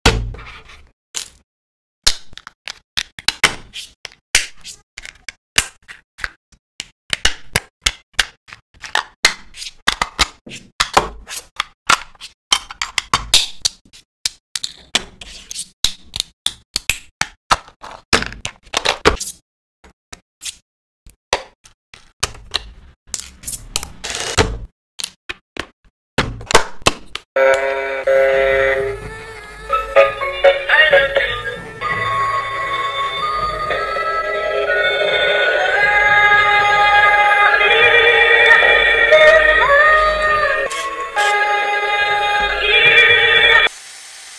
Unbelievable Water Cannon Boat
Hello Everyone,
In this video we lovingly assemble the Toy Water Cannon Boat and testing the fate of the Water Cannon Boat Toy, all this done in ASMR style to blow your mind and completely in speed build.